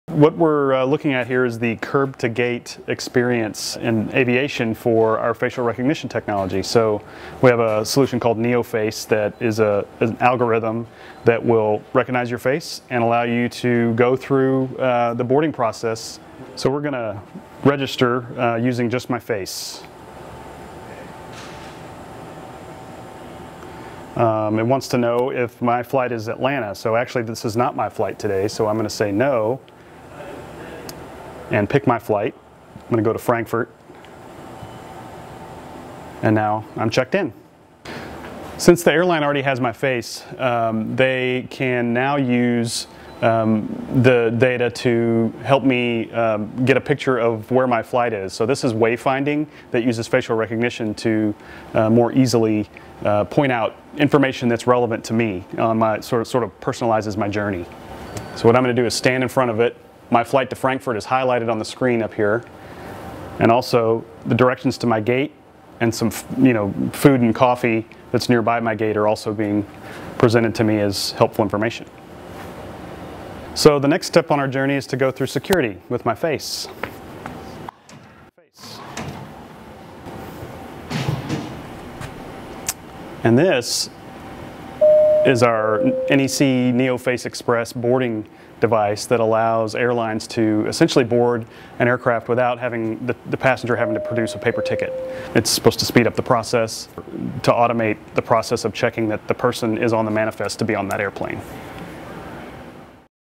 NEC's demonstration of NeoFace Express, a facial recognition system for the airline industry
NEC senior manager of marketing communications demonstrates how the curb-to-gate use of facial recognition makes it easier to board you flight. NeoFace Express is being used for the airline industry. (DMN video/editing: Tom Fox)